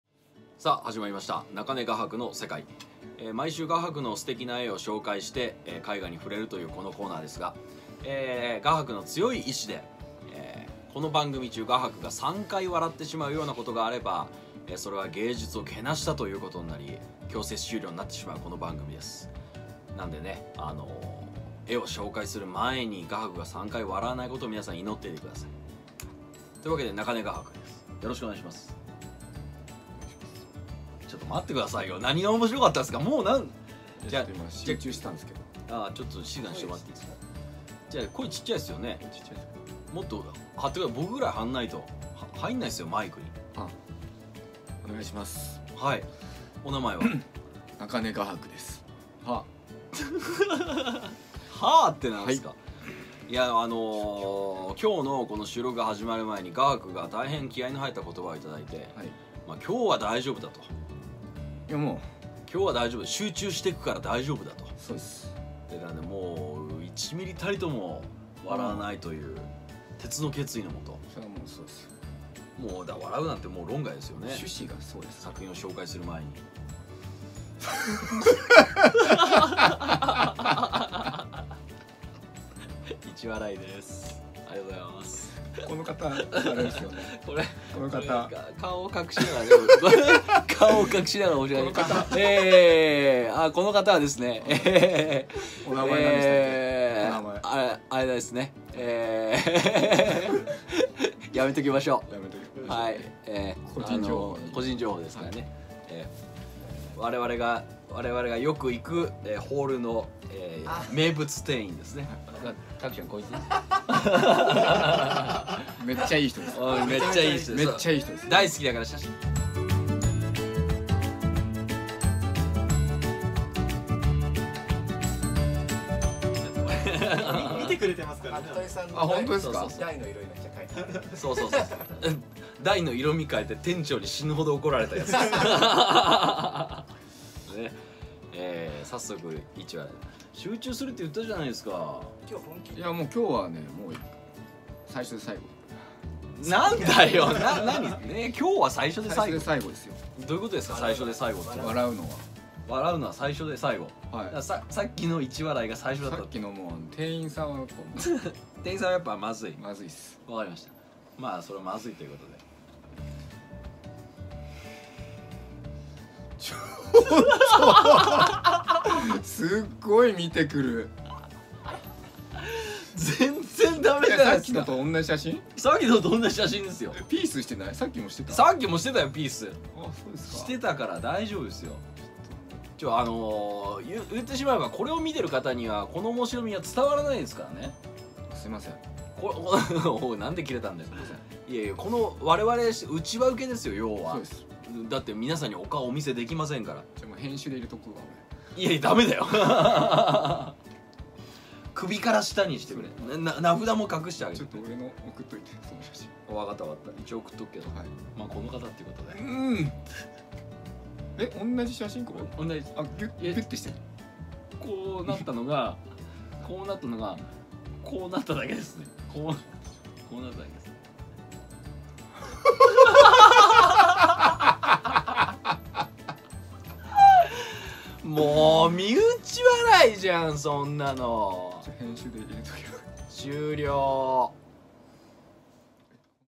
【バラエティー】最早絵にもふれず・・３回笑ったら終了「中根画伯の世界」
中根画伯が３回笑ったら終了のこの番組。最早身内ネタのパチ屋の店員写真にコテンパンにされます。見ている方に伝わらないじゃないっすか・・（泣）